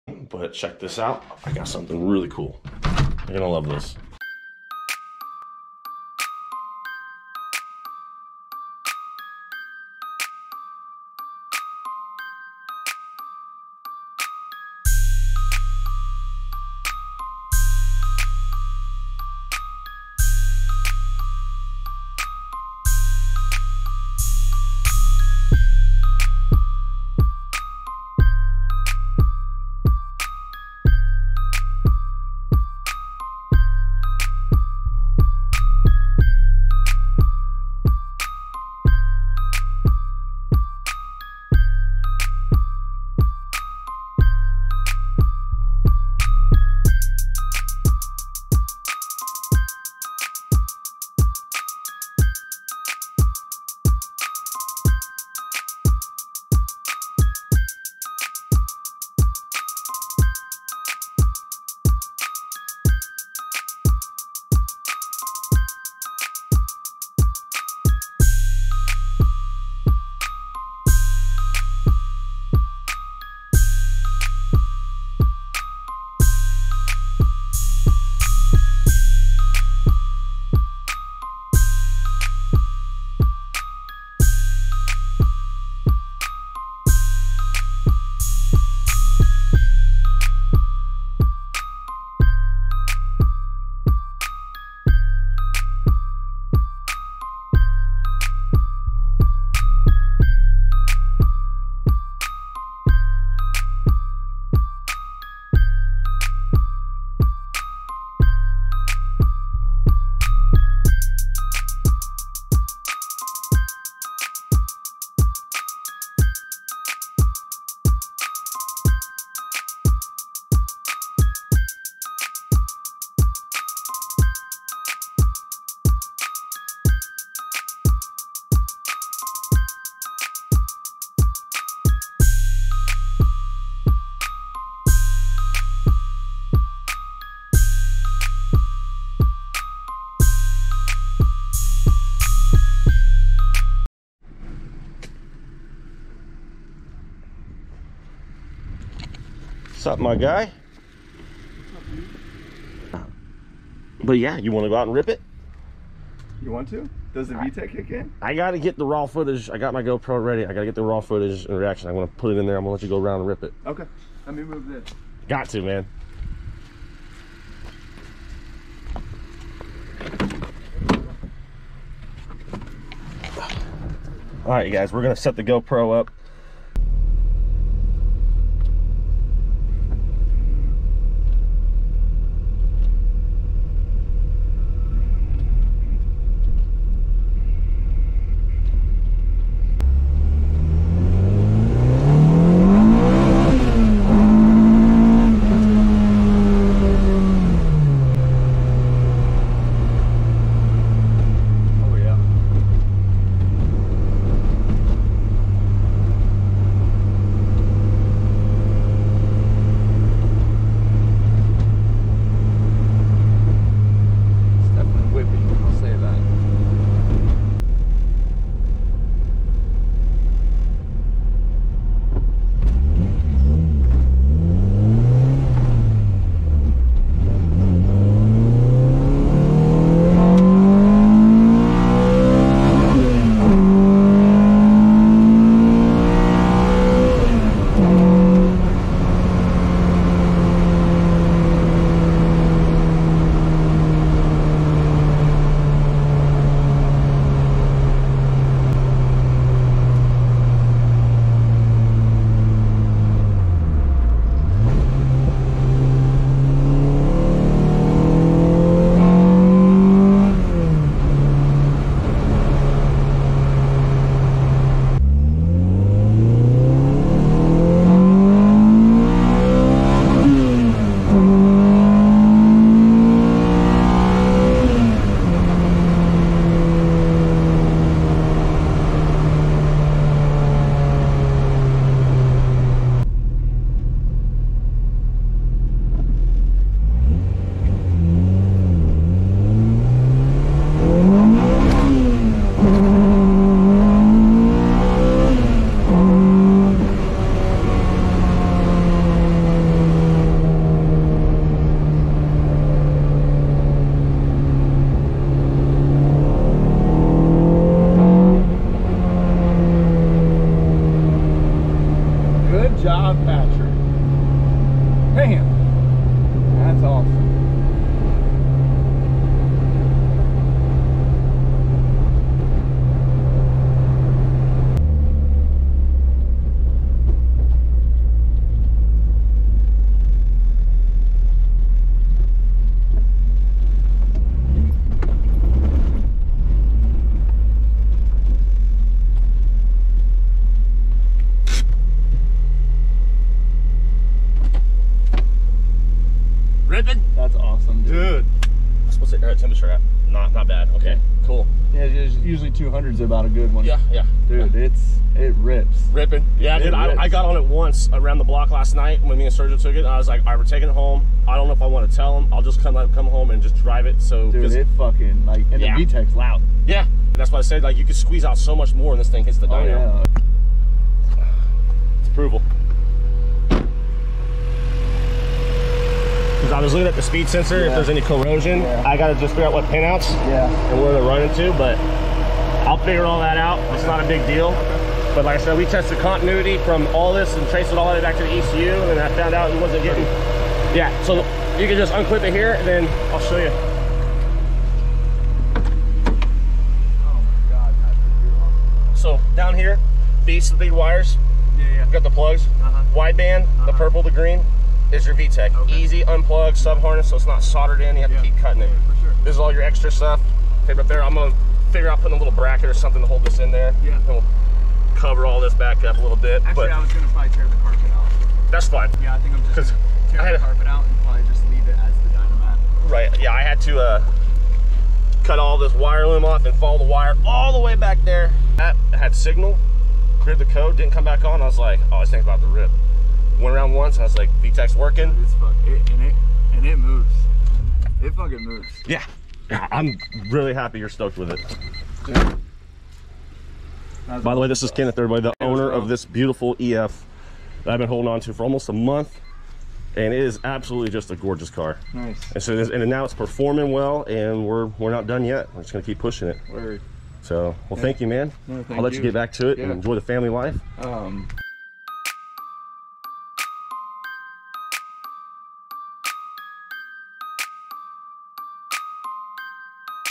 type r rips!.. sat for almost a year after having a bad ECU and NO VTEC. NOW she is ready for more!
civic came in on a trailer! not running. NOW she rips! more to come on this clean EF!